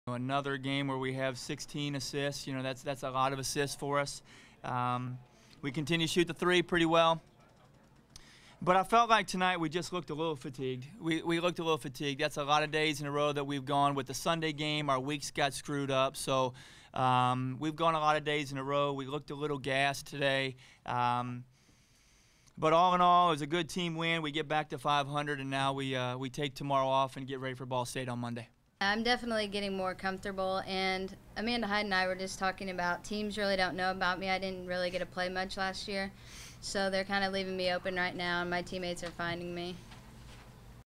11-13-12 Women's Basketball Post Grace
Head Women's Basketball Coach Chris Paul and Sophomore Guard Haley Seibert discuss the Mastodon's 68-52 victory over Grace.